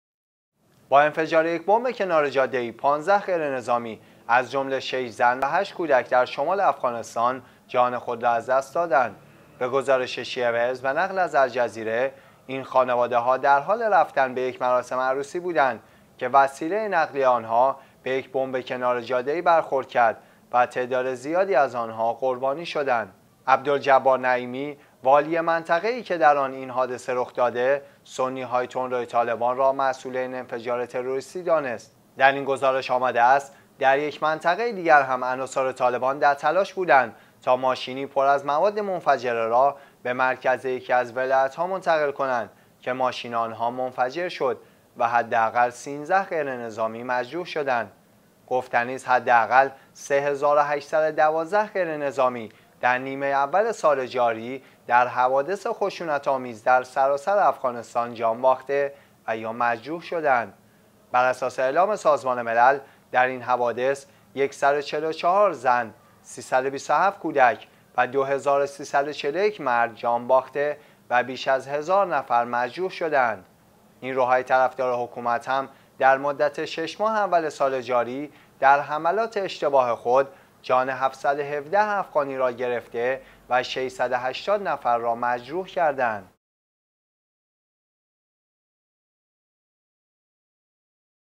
۱۵ غیرنظامی در انفجار بمب کنار جاده ای در شمال افغانستان جان باختند
با انفجار یک بمب کنار جاده ای، ۱۵ غیرنظامی از جمله شش زن و هشت کودک در شمال افغانستان، جان خود را از دست دادند.
به گزارش شیعه ویوز به نقل از الجزیره، این خانواده ها در حال رفتن به یک مراسم عروسی بودند که وسیله نقلیه آنها به یک بمب کنار جاده ای برخورد کرد و تعداد زیادی از آنها قربانی شدند.
عبدالجبار نعیمی، والی منطقه ای که در آن این حادثه رخ داده، سنی های تندروی طالبان را مسئول این انفجار تروریستی دانست.
در این گزارش آمده است، در یک منطقه دیگر هم عناصر طالبان در تلاش بودند تا ماشینی پر از مواد منفجره را به مرکز یکی از ولایت ها منتقل کنند که ماشین آنها منفجر شد و حداقل ۱۳ غیرنظامی مجروح شدند.
گفتنی است، حداقل ۳،۸۱۲ غیرنظامی در نیمه اول سال جاری در حوادث خشونت آمیز در سراسر افغانستان جان باخته و یا مجروح شده اند. بر اساس اعلام سازمان ملل، در این حوادث ۱۴۴ زن، ۳۲۷ کودک و 2341 مرد، جان باخته و بیش از ۱۰۰۰ نفر مجروح شده اند.
نیروهای طرفدار حکومت هم در مدت شش ماه اول سال جاری در حملات اشتباه خود، جان ۷۱۷ افغانی را گرفته  و ۶۸۰ نفر را مجروح کردند.